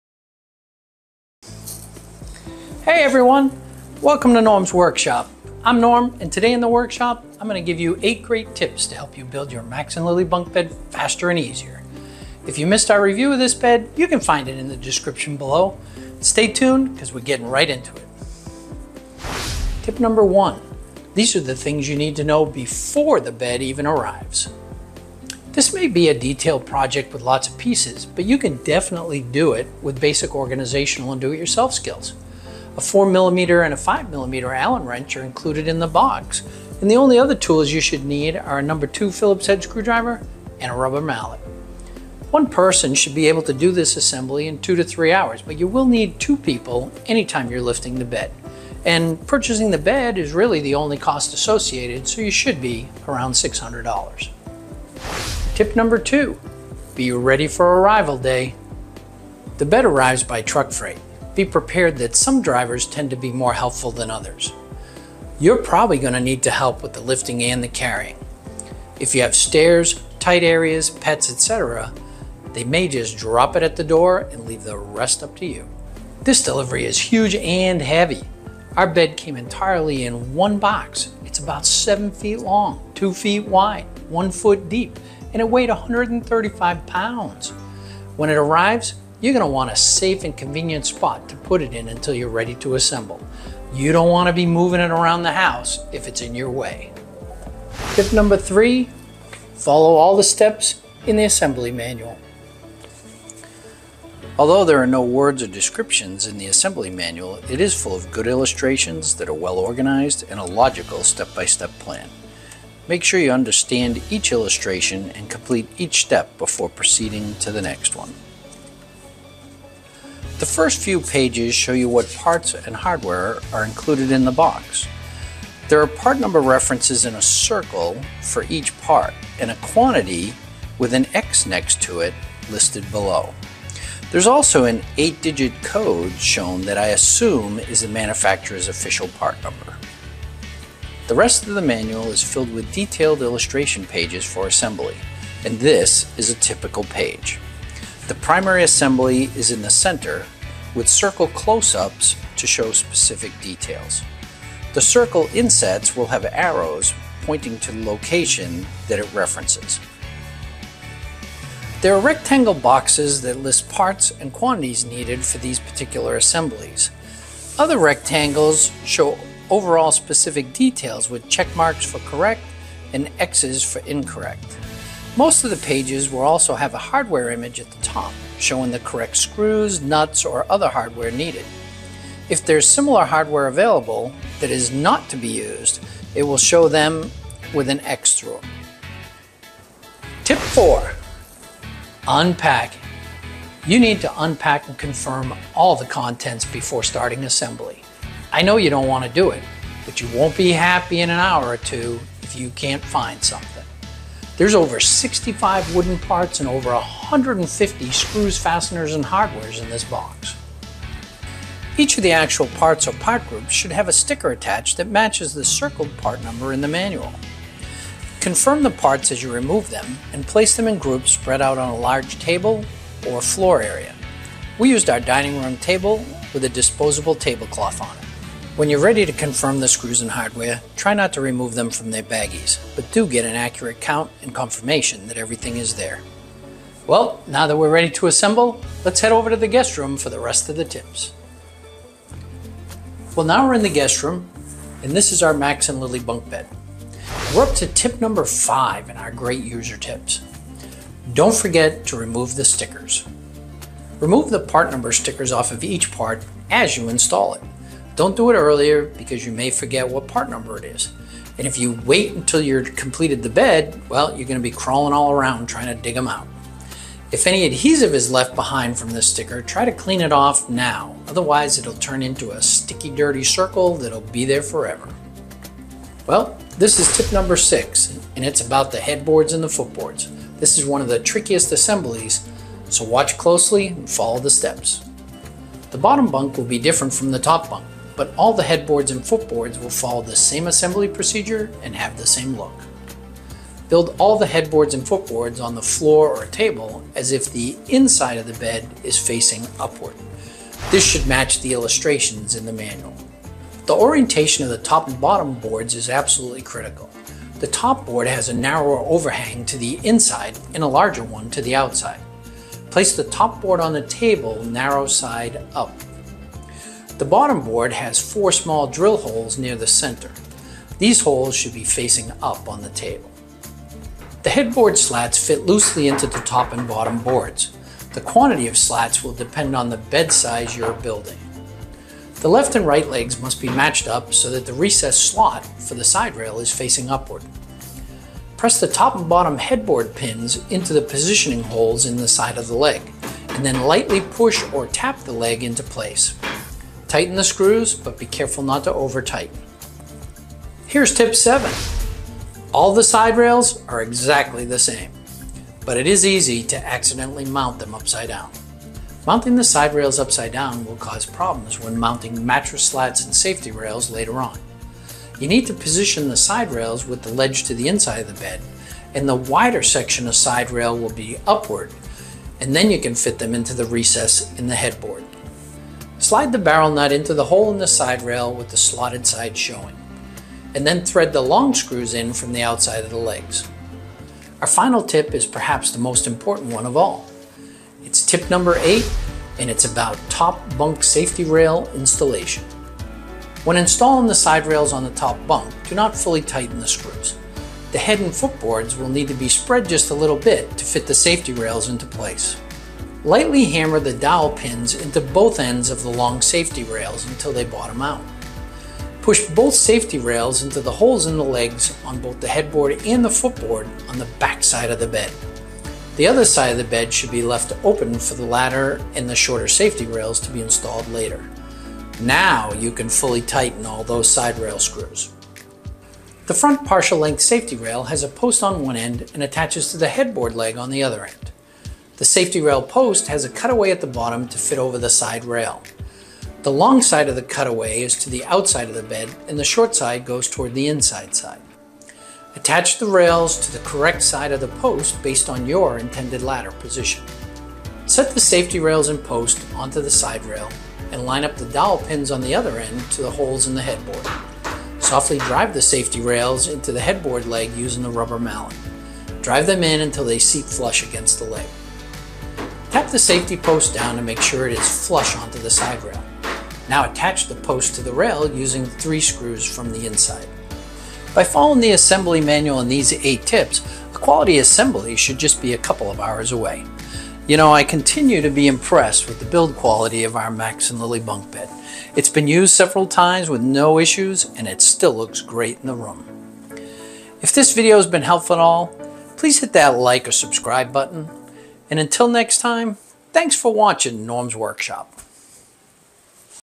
8 Great Tips To Build Your Max & Lily Bunk Bed
Here are 8 great tips to make your Max & Lily Bunk Bed assembly faster and easier. These tips don’t replace the assembly instructions but they sure make them easier to follow. The Max & Lily Bunk Bed is Ready To Assemble furniture and it will take hours to complete the assembly. By following the tips you will reduce the stress and shorten the time needed to build the bed.

Don’t Buy A Bunk Bed Until You Check Out

FEATURED ITEMS:
Max & Lily Bunk Bed

Chapters
00:00 Norm’s Workshop
00:19 Tip #1 Before the Bed Arrives
00:54 Tip #2 Arrival Day
01:37 Tip #3 Follow the Steps
03:09 Tip #4 Unpacking
04:01 Tip #5 Remove the Stickers
04:35 Tip #6 Headboards & Footboards
05:50 Tip #7 Siderails
06:23 Tip #8 Safety Rails
07:50 Final Thoughts